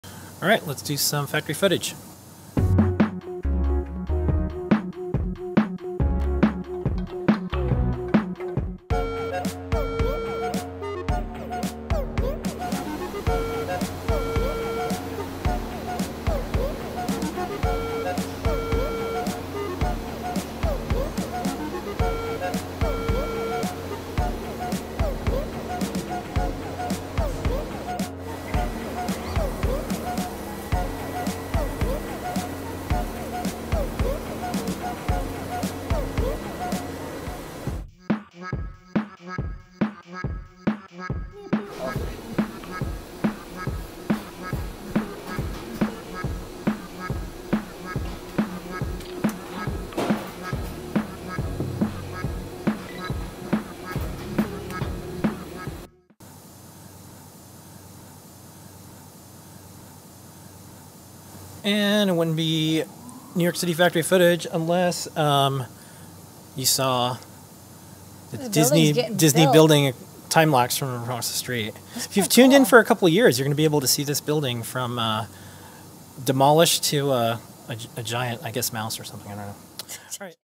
Made in NYC 9/15/2021 Featuring Adafruit CYBERDECK and LED Glasses
LED Glasses - will you get bug, cat, wolf or dragon? (0:02)

Manufacturing CYBERDECK HATs (0:10)

Testing LED Glasses (0:39)

Reels before and after (0:57)

Ever rising construction (1:02)